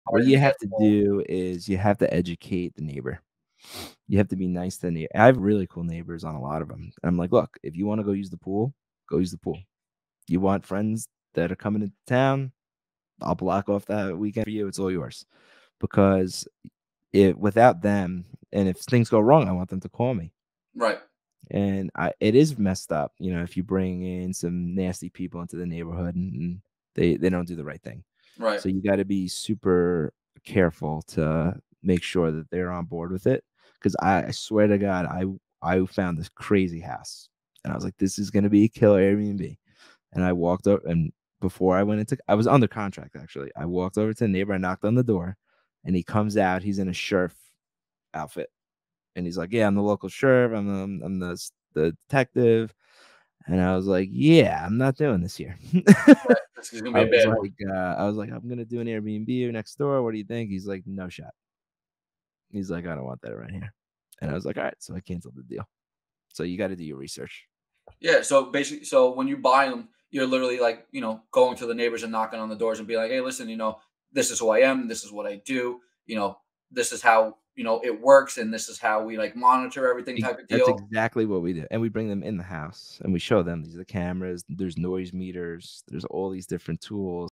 AirBnb and Neighbor Issues
Starting up an AirBnb but worried about the neighbors? 🤔 Follow these tips to make friends and remain friends with the neighbors! 🙌